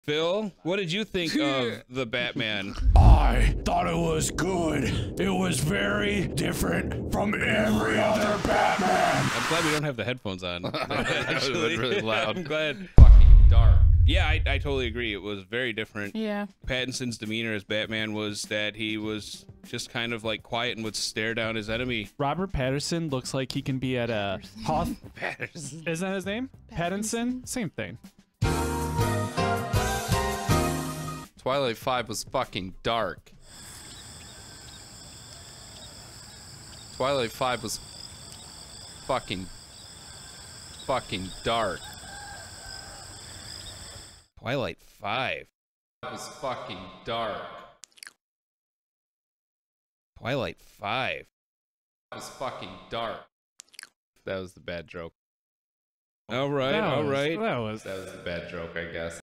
The Batman Clip with Phil and Chris
Phil and Chris talk about The Batman briefly Mike Reardon Reviews